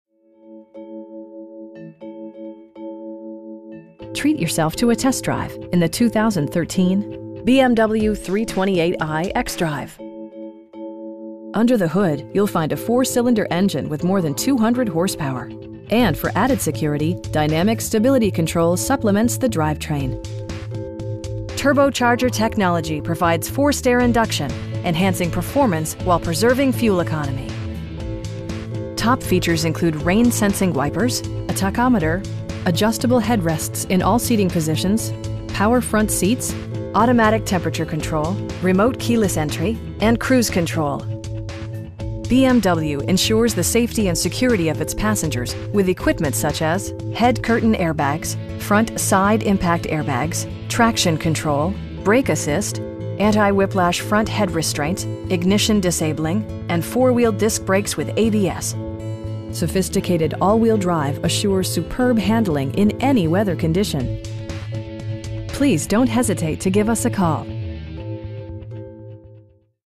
2013 BMW 328i xDrive w/SULEV in Dedham, MA 02026
Treat yourself to a test drive in the 2013 BMW 328i xDrive. Under the hood you'll find a 4 cylinder engine with more than 200 horsepower, and for added security, dynamic Stability Control supplements the drivetrain. Turbocharger technology provides forced air induction, enhancing performance while preserving fuel economy. Top features include rain sensing wipers, a tachometer, adjustable headrests in all seating positions, power front seats, front bucket seats, automatic temperature control, remote keyless entry, and cruise control. BMW ensures the safety and security of its passengers with equipment such as: head curtain airbags, front side impact airbags, traction control, brake assist, anti-whiplash front head restraints, ignition disabling, and 4 wheel disc brakes with ABS. Sophisticated all wheel drive assures superb handling in any weather condition. Our aim is to provide our customers with the best prices and service at all times. Please don't hesitate to give us a call.